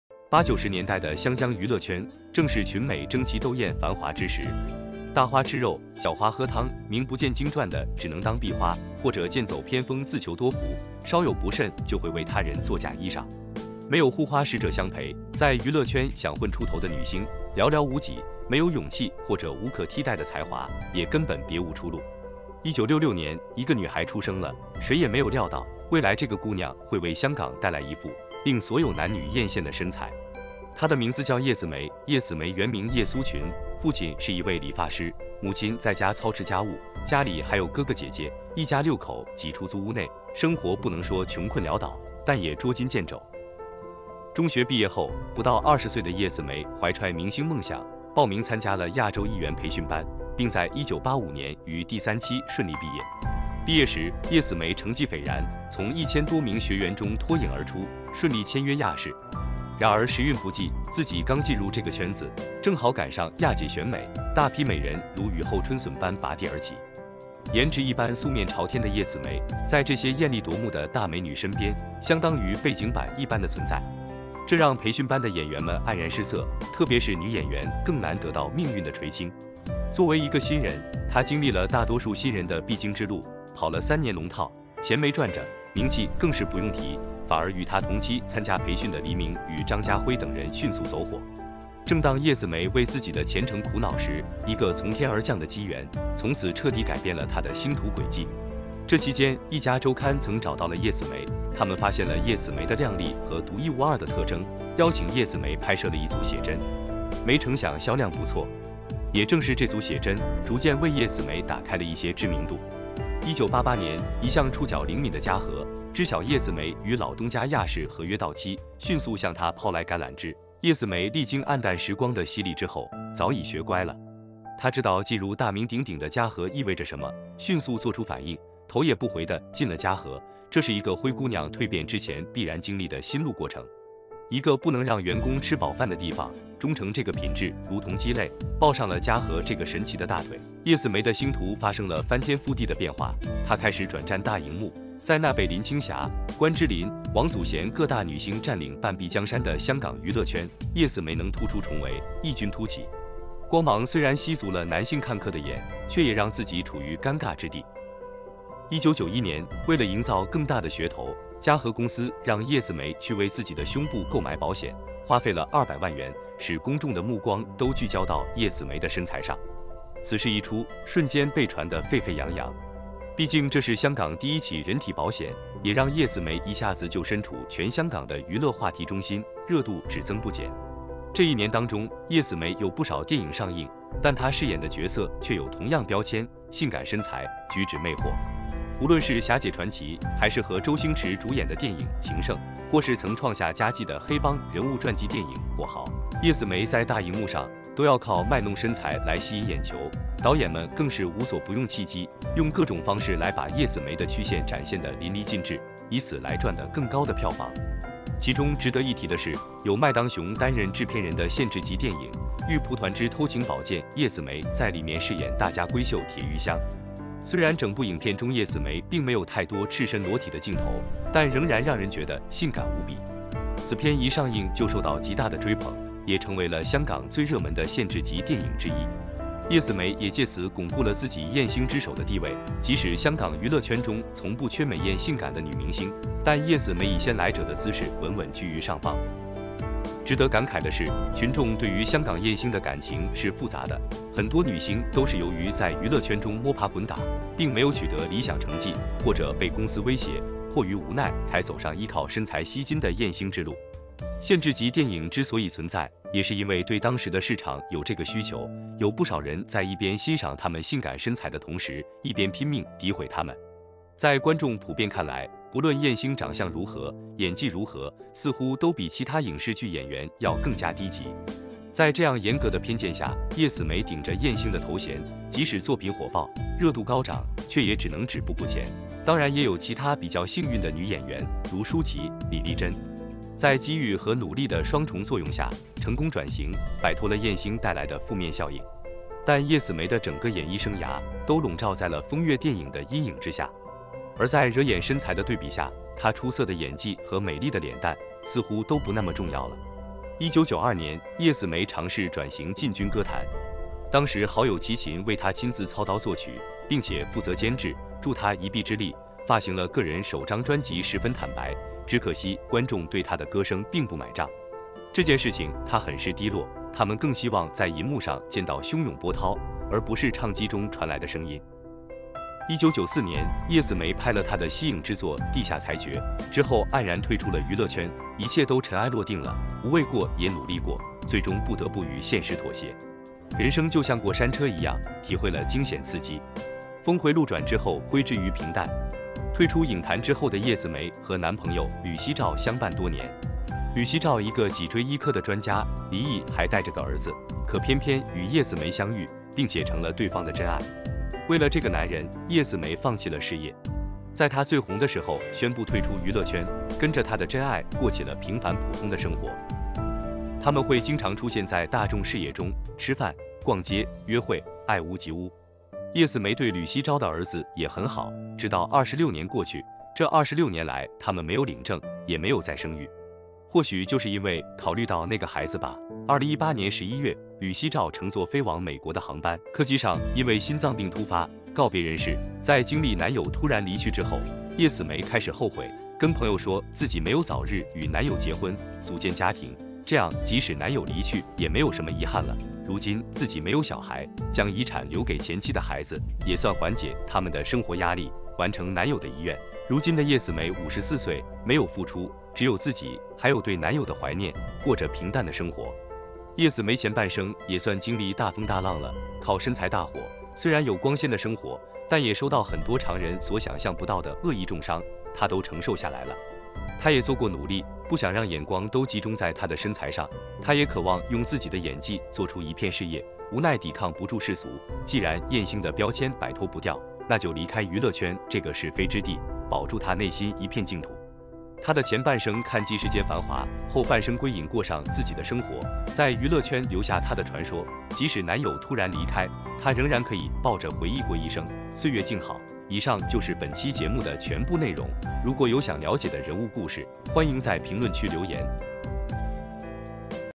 “性感波神”叶子楣：花两百万为身体上保险，晚年却孤身一人。
叶子楣前半生也算经历大风大浪了，靠身材大火，虽然有光鲜的生活，但也收到很多常人所想像不到的恶意中伤，她都承受下来了。
她也做过努力，不想让眼光都集中在她的身材上，
她也渴望用自己的演技做出一片事业，无奈抵抗不住世俗，
既然"艳星"的标签摆脱不掉，那就离开娱乐圈这个是非之地，保住她内心一片净土。
她的前半生看尽世间繁华，后半生归隐过上自己的生活，在娱乐圈留下她的传说，
即使男友突然离开，她仍然可以抱着回忆过一生，岁月静好。